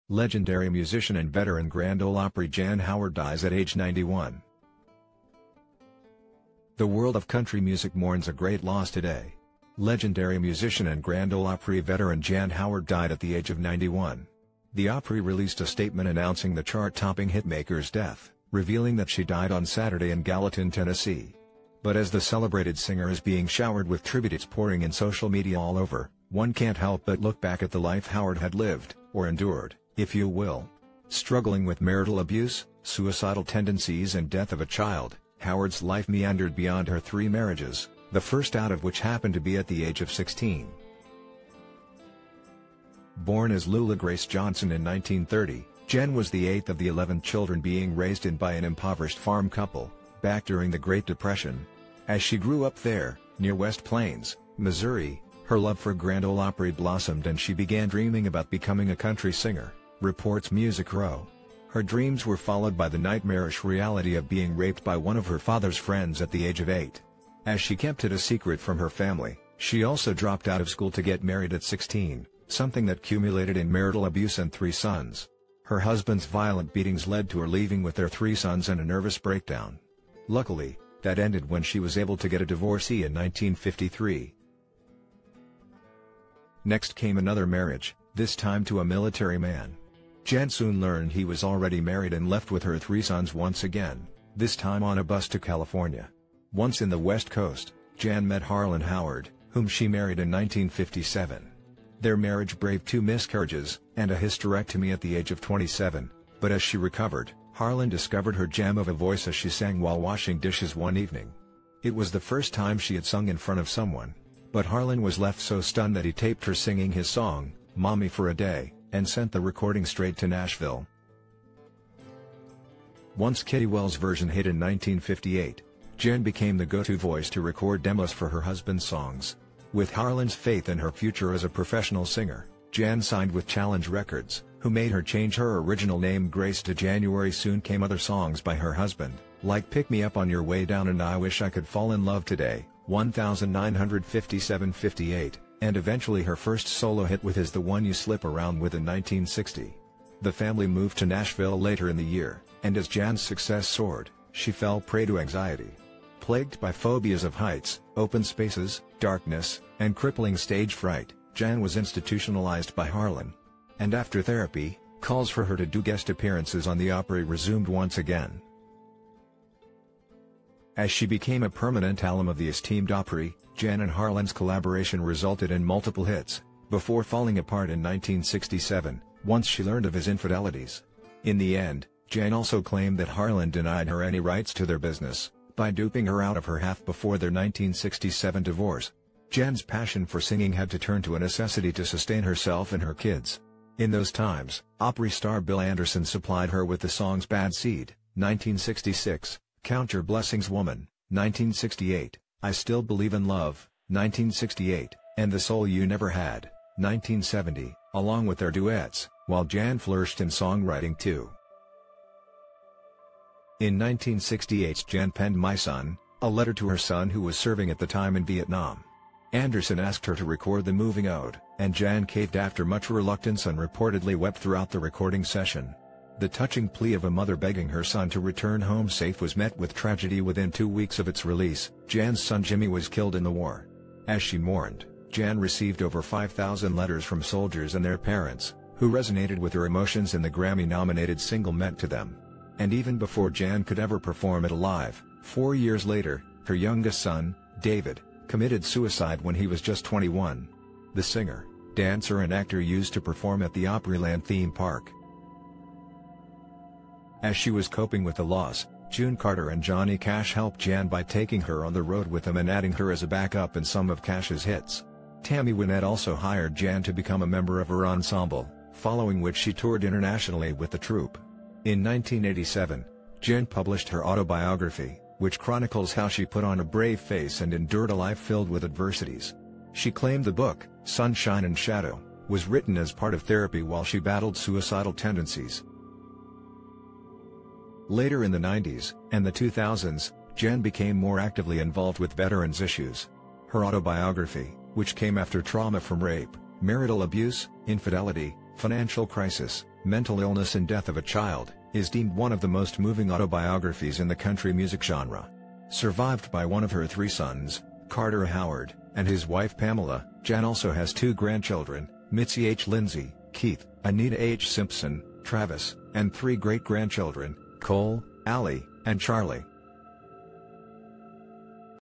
Legendary musician and veteran Grand Ole Opry Jan Howard dies at age 91
Grand Ole Opry
Jan Howard